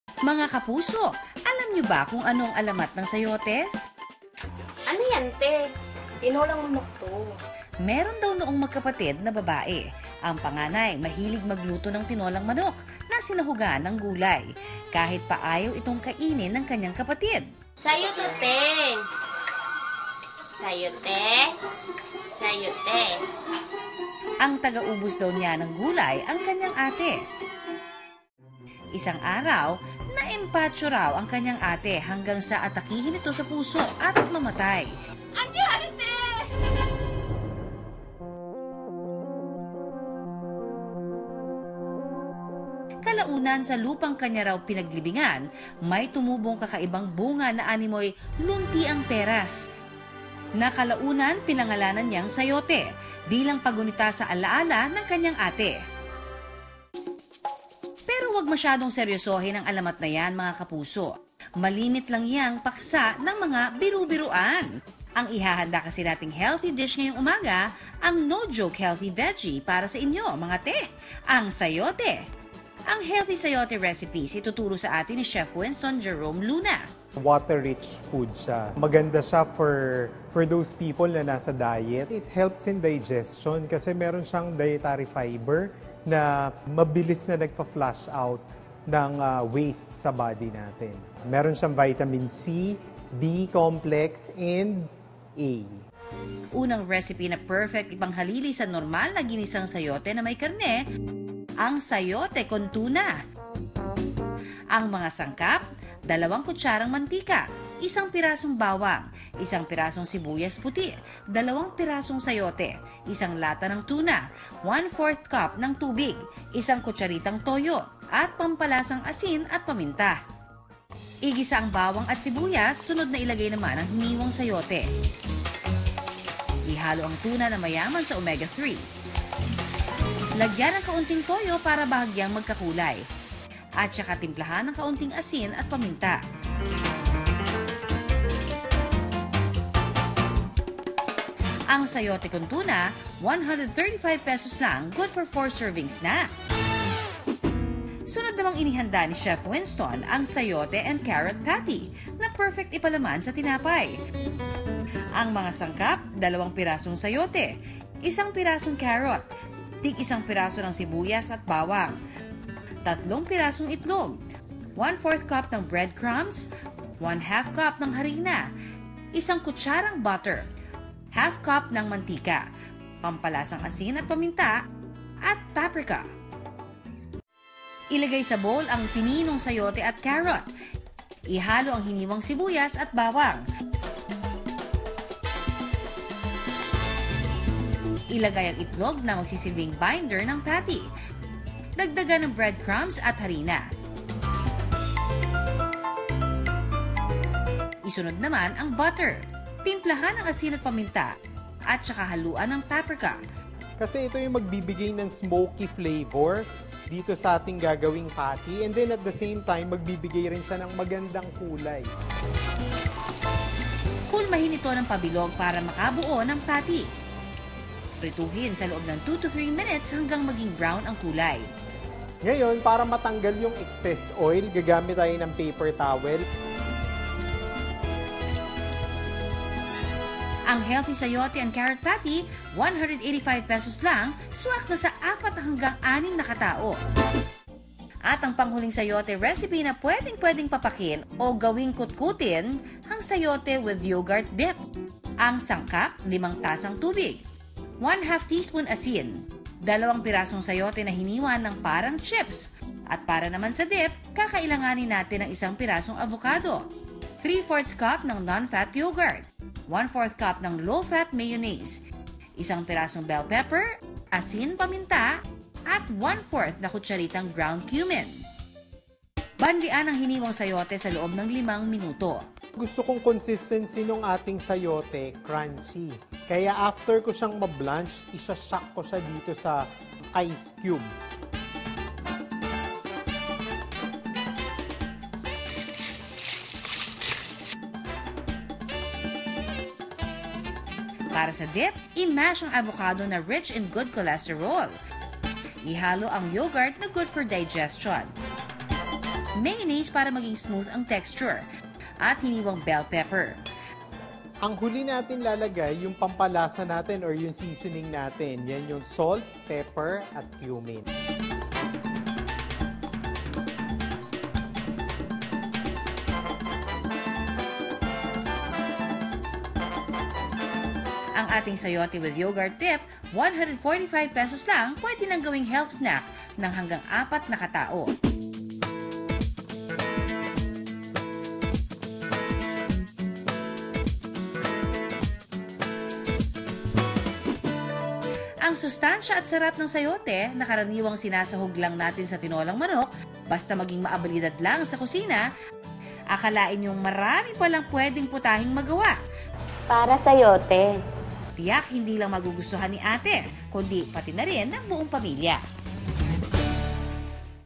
Pinoy MD: Busog-lusog sayote recipes
Ngayong umaga, hindi lamang mabubusog ang inyong tiyan, mabubusog din kayo sa kaalaman ukol sa iba’t ibang putahe ng sayote na magugustuhan ng inyong pamilya. Kung ano-ano iyan, panoorin sa video na ito.